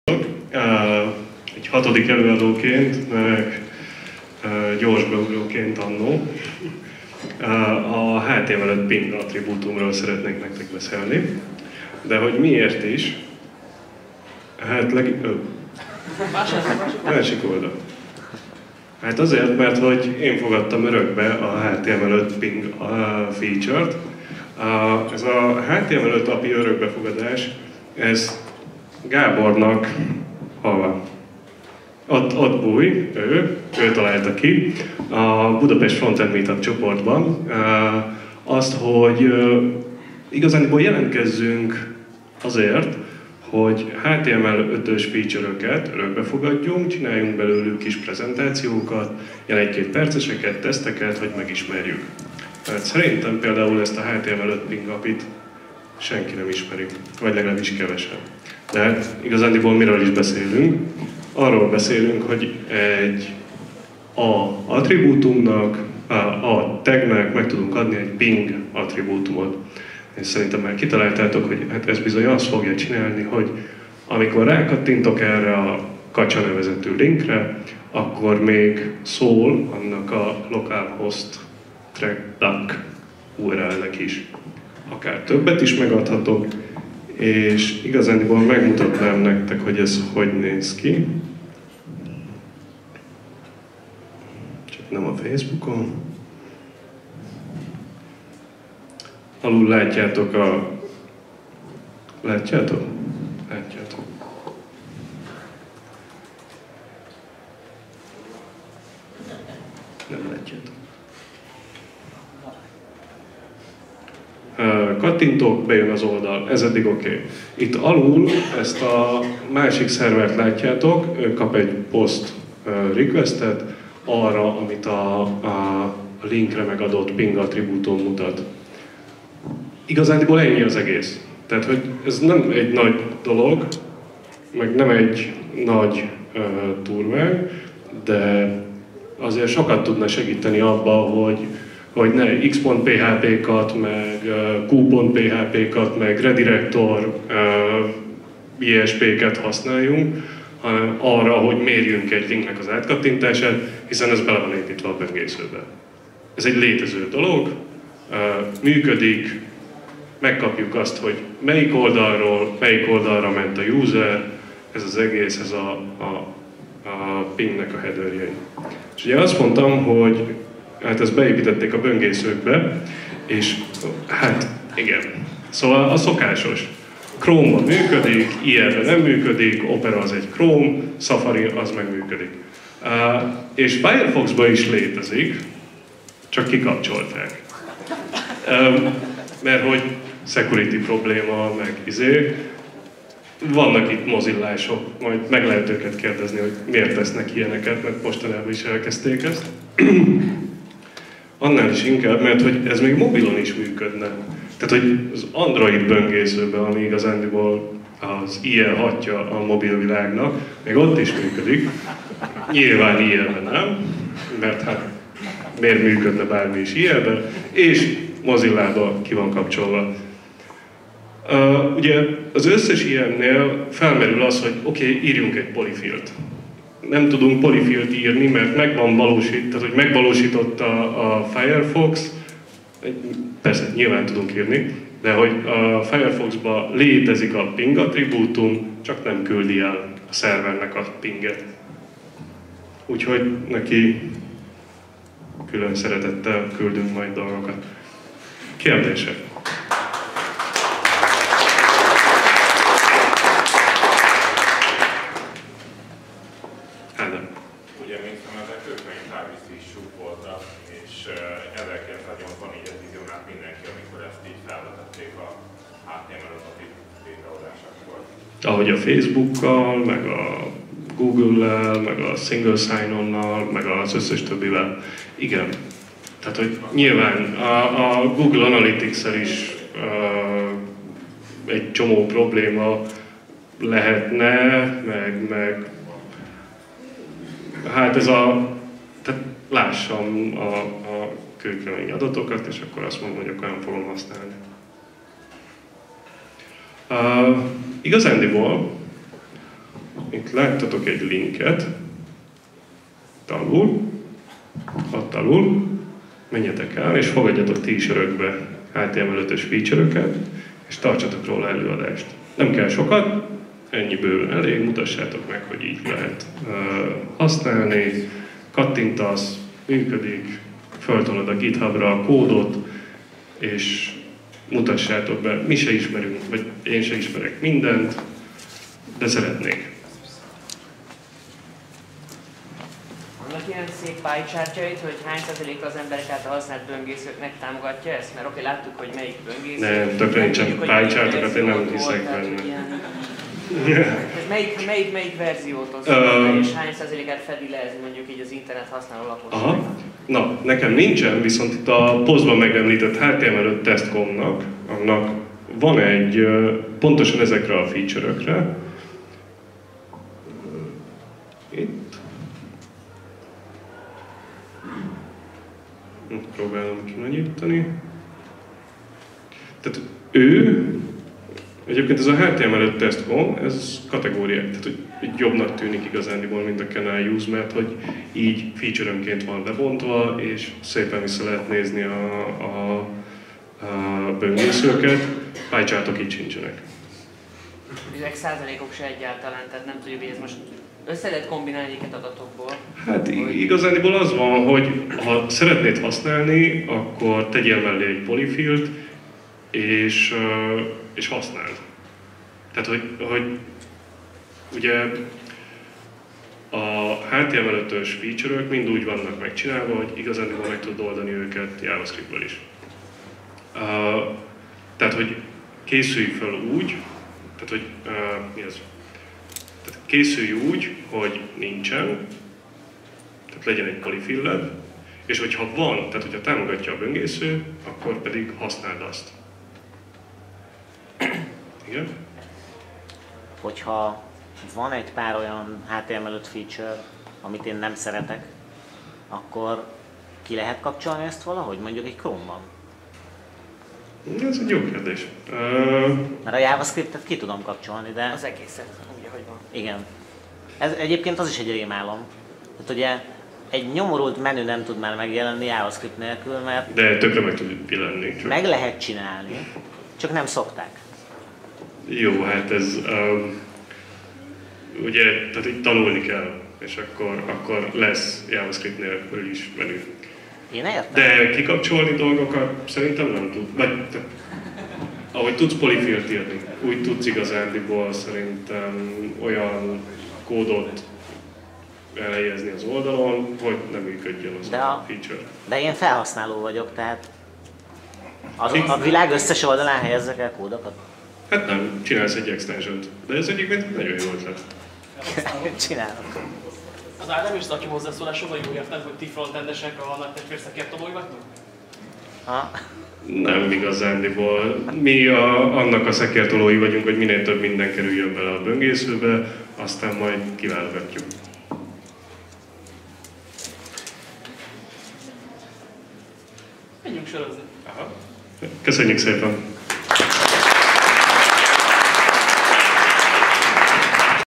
Szalóki Róbert (EUedge): HTML5 örökbefogadás, a HTML5 ping
Az előadás a Budapest New Technology Meetup előadássorozaton hangzott el 2015. január hónapban. A közösség honlapja